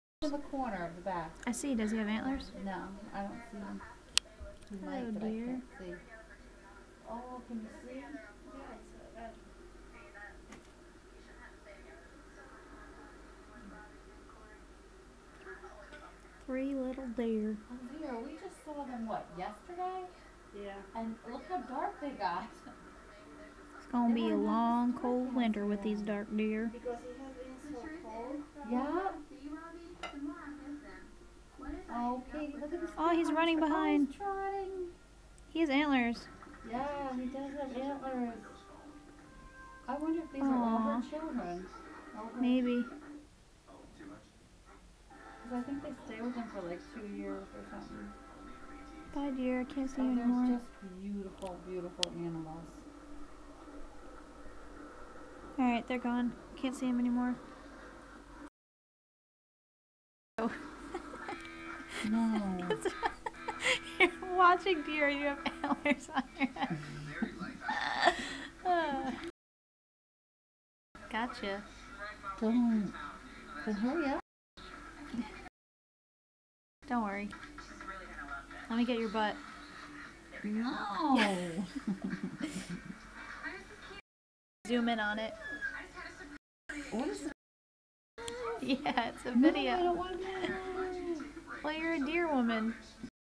Mommy the deer
This is my mother [ kathryn] and i watching deer pass through a friend's backyard. THERE'S A CRAZY TWIST AT THEE END SO WATCH THE WHOLE THING